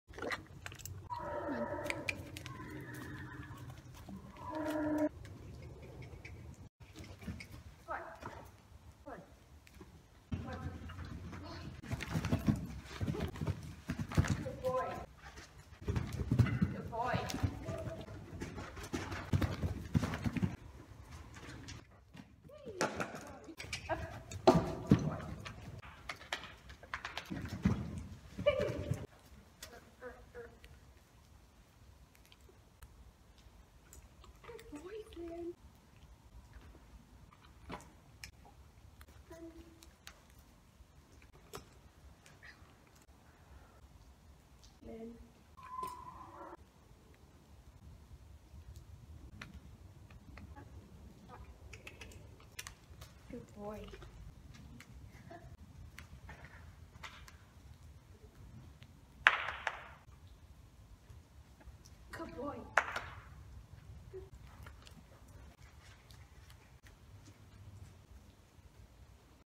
BAREBACK HORSE RIDING- Trick Riding, Jumping and a Fall
Just me n Flynn messin' around

Flynn:
11yo Welsh Cob section D, dark bay 15hh
Owned for 6 years, ive been riding him for 3 years (hes my sisters lol)

Me:
Lucy, 16yo gal
Socials:
Horse instagram: @flynnjumping
Personal instagram: @_lucy_morgan_